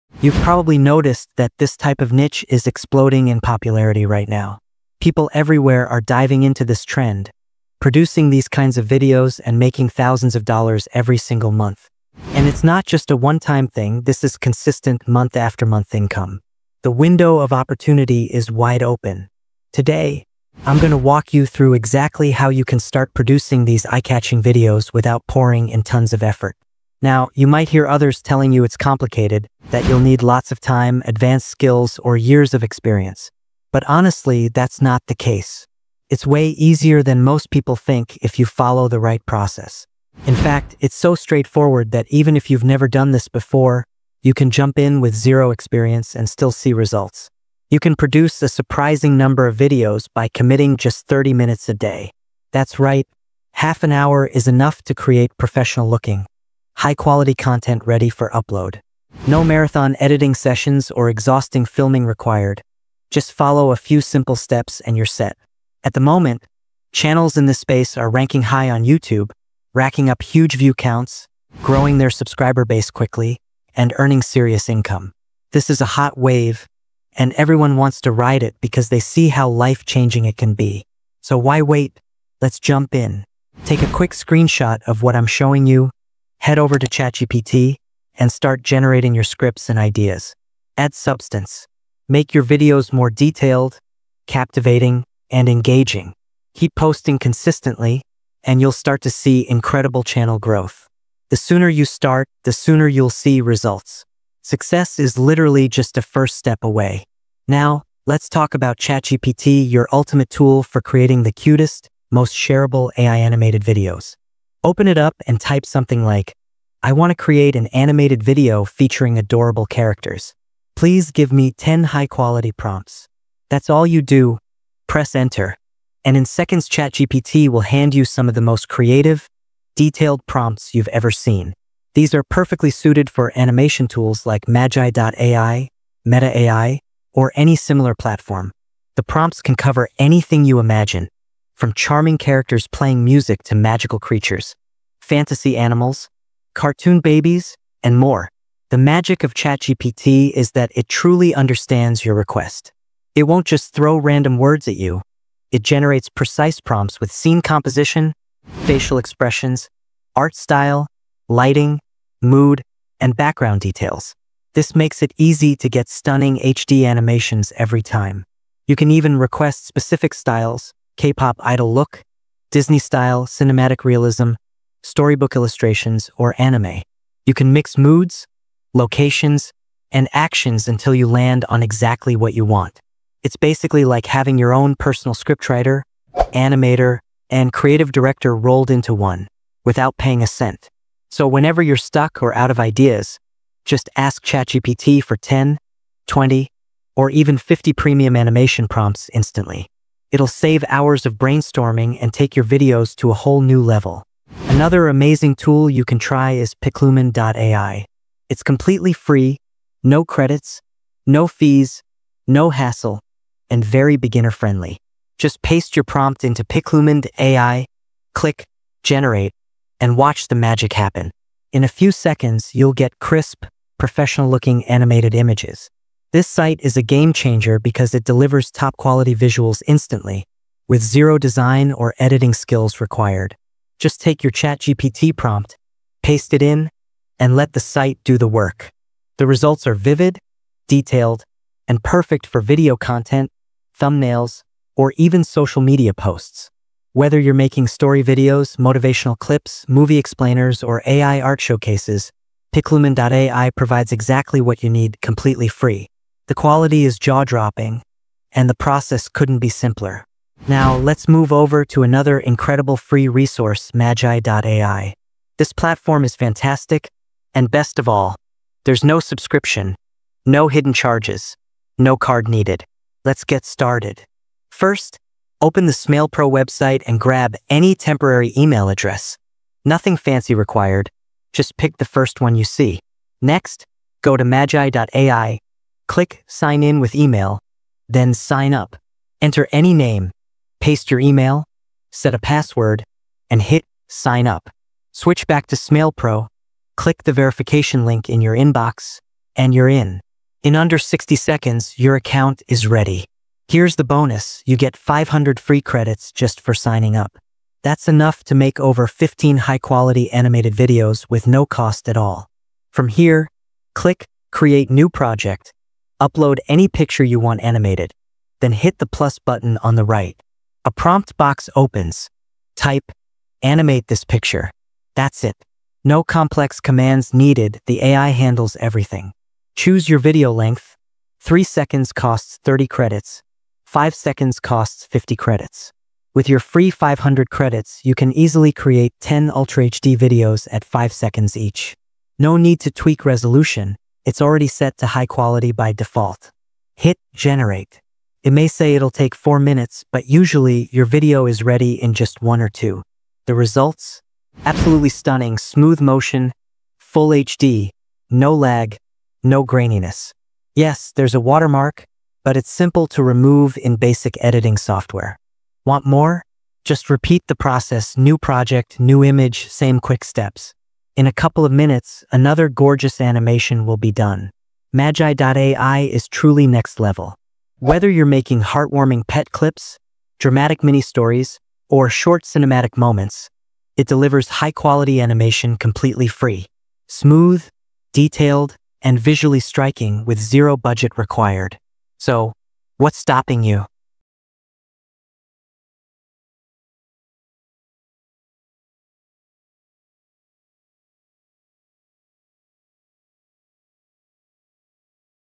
This New AI tool will change your life – No Credit Card, No Tricks! You’ll Love It!
🎥 Title: 1000% FREE AI Video & Image Generator | Your Animated Life is About to Change Forever!

📌 Description:

Unlock the power of AI without spending a dime! In this step-by-step tutorial, I’ll show you how to generate stunning, cinematic-quality AI images and animated videos using 100% FREE tools — no credit card, no watermarks, no limits.

We’ll walk through:

✅ VPN + CapCut Video → Unlock free video generation (Model 2.0 for fast, high-quality results)
✅ CapCut editing → Transitions, effects, on-screen text, and perfect timing

🎁 By the end, you’ll have:

✨ A polished, HD, watermark-free animated video
✨ Full creative control using free AI tools
✨ The power to elevate your content creation game

✏️ ChatGPT Prompt Templates & Scripts
📥 Get them inside the video!

💬 Got questions or want a specific tutorial? Drop a comment — I reply fast!